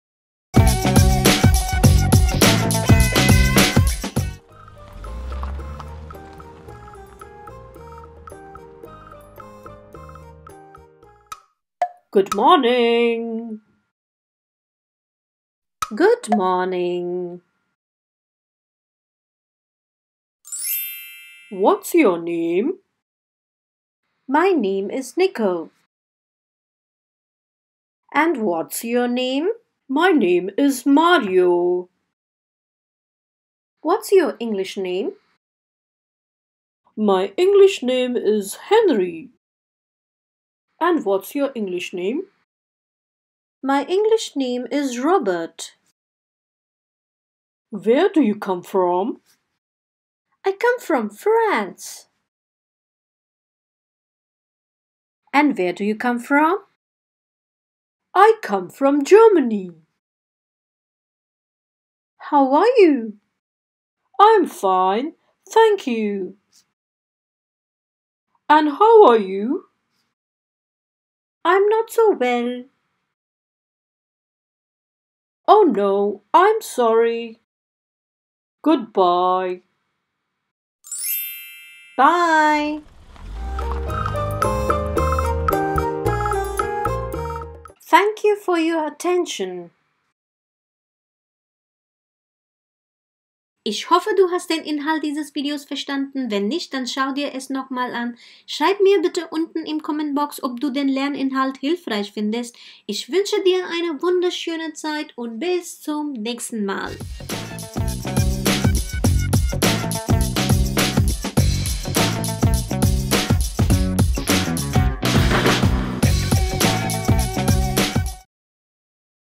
[Greeting] How are you? I That' me I Teil 1 I Grundschule 3. Klasse I Englisch
Herzlich Willkommen zu meinem Video.

In diesem Video erkläre ich den Schülern der 3. Klasse, wie sie sich auf Englisch vorstellen können. Wir lernen gemeinsam, wie man sich begrüßt, wie man fragt, wie es jemandem geht, und wie man über sich selbst spricht.

Schau dir mein Video an, um das Thema "Greeting: How are you? That's me!" zu verstehen und spielerisch Englisch zu lernen.

Viel Spaß dabei! Ich freue mich sehr auf ein Like, wenn dir dieses Video gefällt.

Liebe Grüße
Lernrad